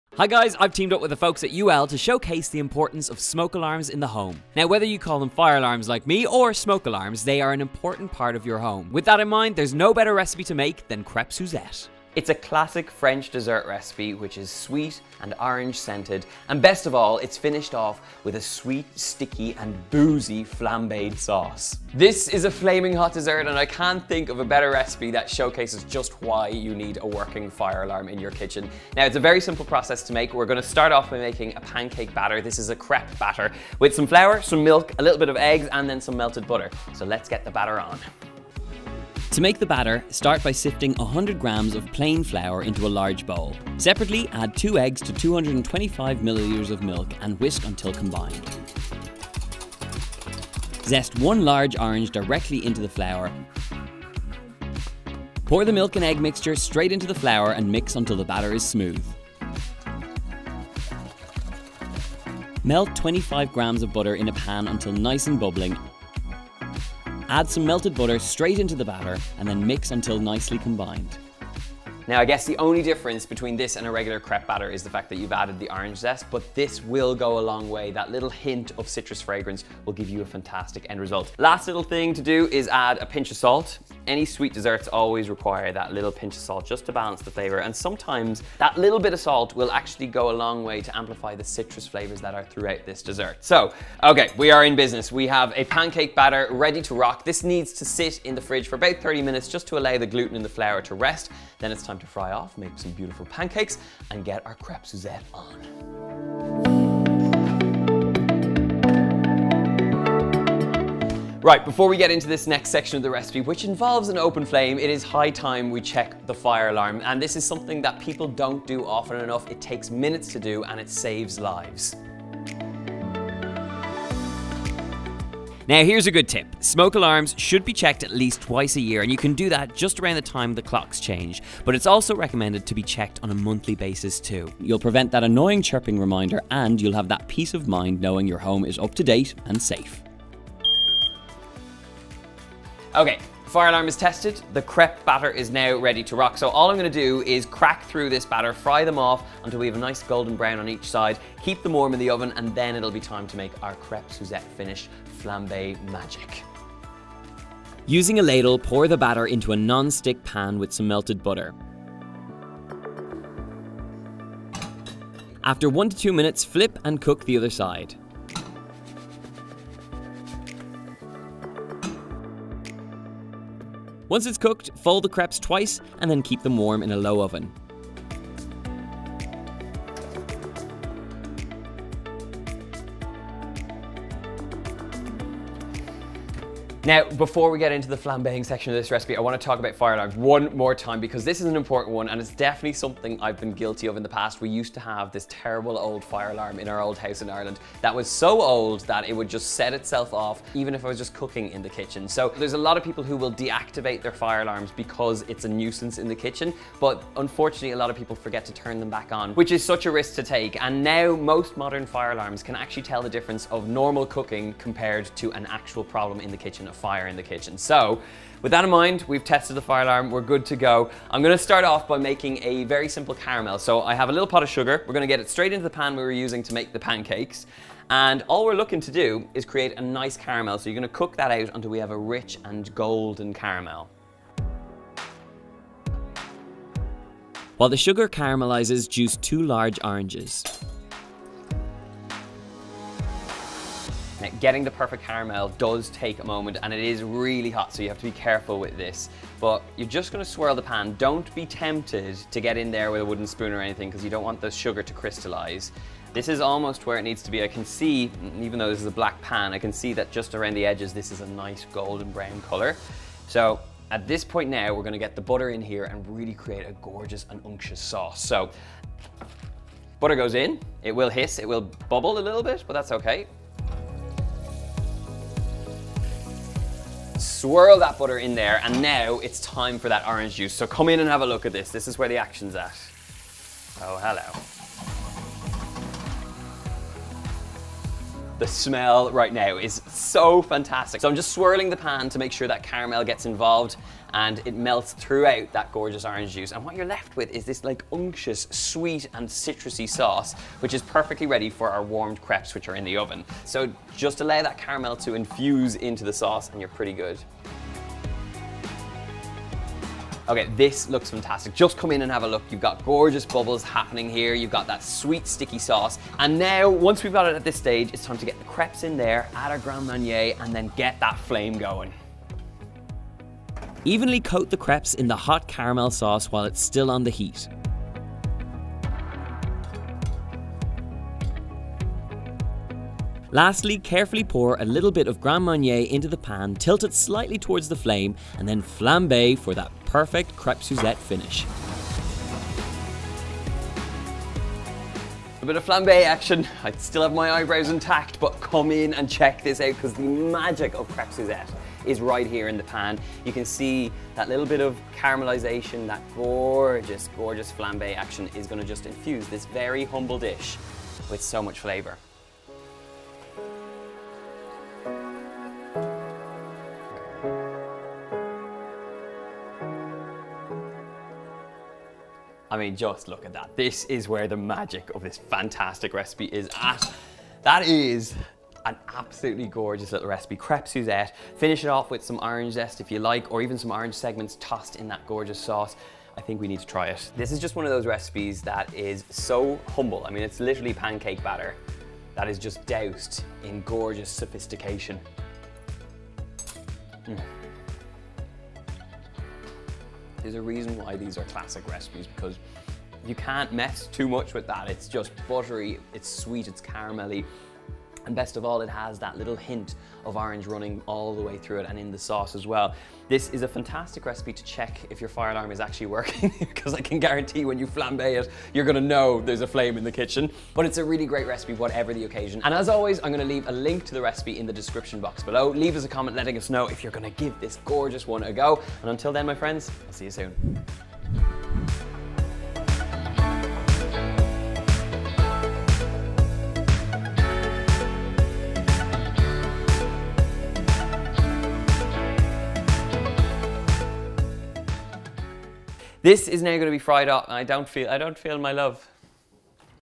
FLAMING HOT PANCAKES?! The Pancake Recipe You Have To Make! Crepes Suzette!
AD | CREPES SUZETTE - An absolute classic dessert recipe that everyone should learn how to make!

I’ve teamed up with the UL Firefighter Safety Research Institute to bring you a mouthwatering French Crepes Suzette recipe! Thin pancakes cooked in a sticky and sweet orange caramel sauce and finished with a flambe of Grand Marnier for added drama! This is definitely the perfect recipe to highlight how important it is to have working smoke alarms in your house. Make sure to check out this video on how to make this simply decadent dessert!